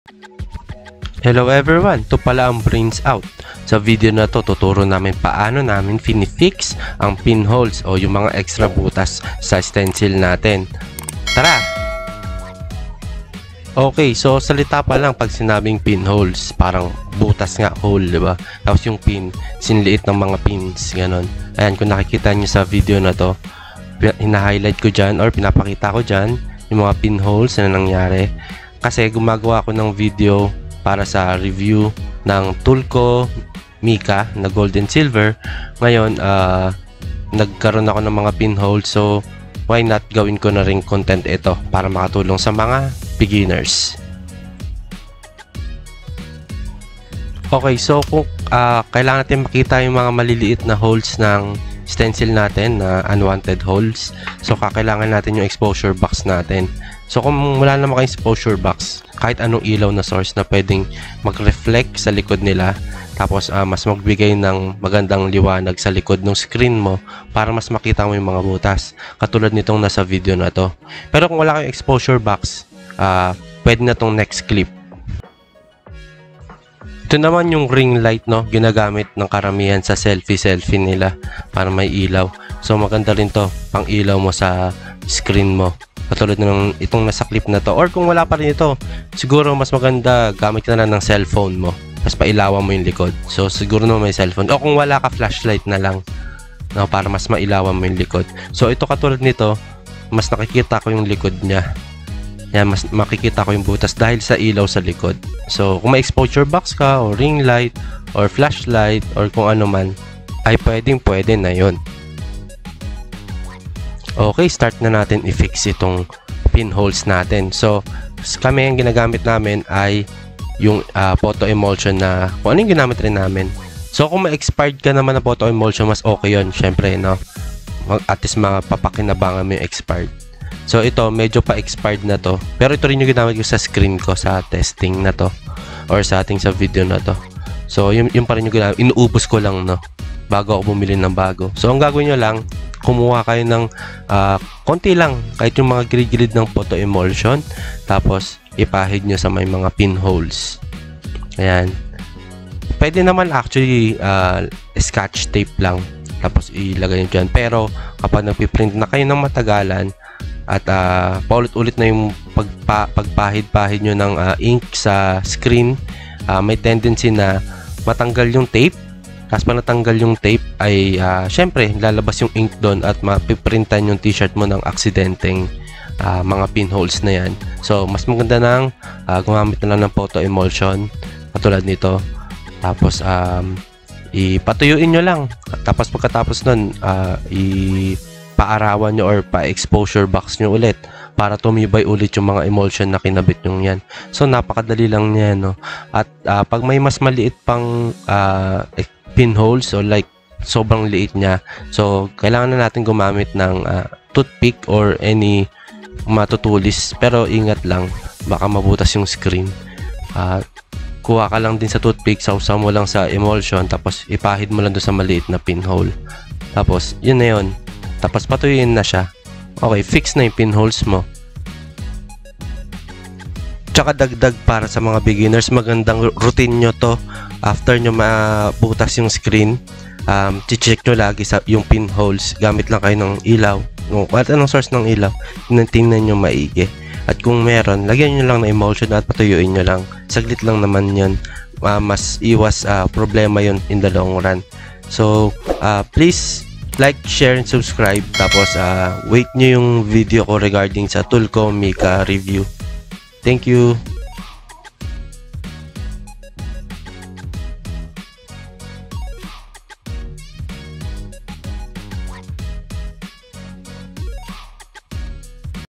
Pinholes - Easy Fix | Screen Printing
In this video we will explain how to easily fix pinholes in your screen. This is a screen printing tutorial for beginners.